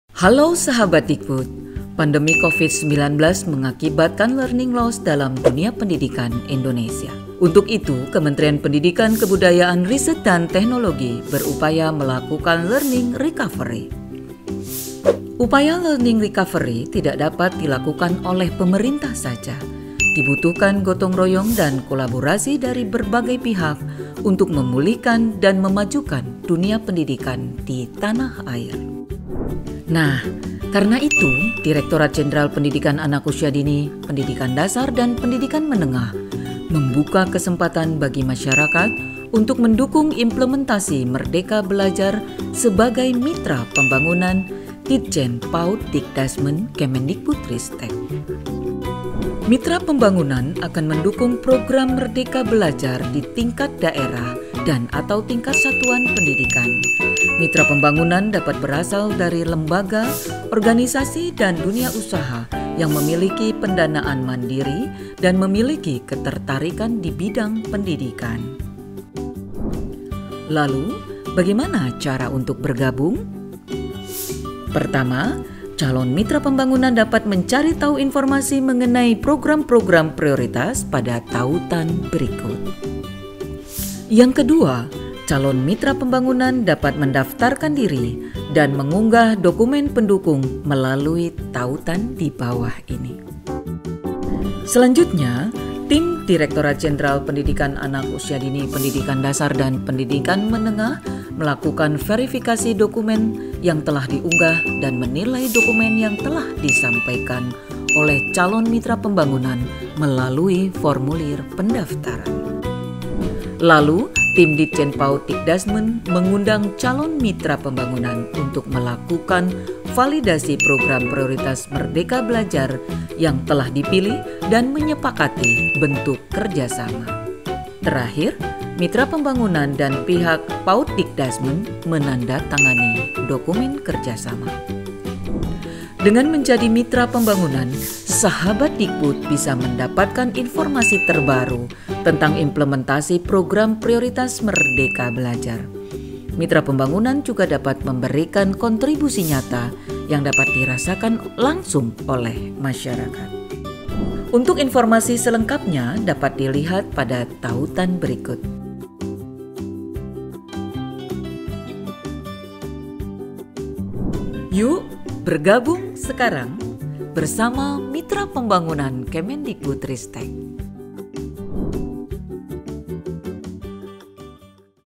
Mitra Pembangunan Ditjen Pauddikdasmen
Pandemi covid-19 menciptakan learning loss untuk pendidikan di Indonesia. Untuk itu Kemendikbudristek berupaya melakukan learning recovery. Upaya learning recovery tidak dapat dilakukan oleh pemerintah saja, dibutuhkan gotong-royong dan kolaborasi dari berbagai pihak untuk memulihkan dan memajukan dunia pendidikan di tanah air.

Oleh karena itu, Direktorat Jenderal Pendidikan Anak Usia Dini, Pendidikan Dasar, dan Pendidikan Menengah membuka kesempatan bagi masyarakat untuk mendukung implementasi merdeka belajar sebagai mitra pembangunan Ditjen Pauddikdasmen Kemendikbudritek